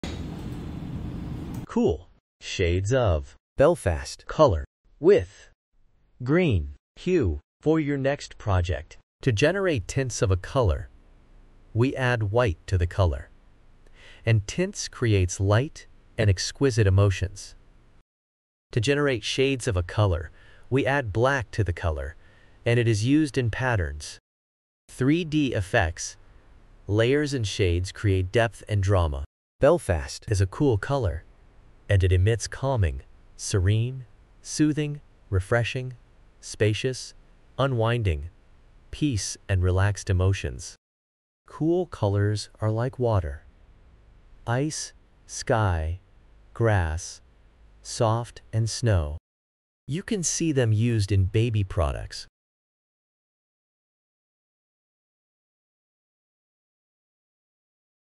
Shades & Tints of Belfast color 558d4f A Cool Green color 4b7d46 426d3d #679962
Shades of Belfast color 558d4f Cool Shades of Belfast color with Green hue for your project!

Shades
Hex 4b7d46

Tints
Hex 679962

Belfast is a cool color, and it emits calming, serene, soothing, refreshing, spacious, unwinding, peace and relaxed emotions. Cool colors are like water, ice, sky, grass, soft and snow. You can see them used in baby products.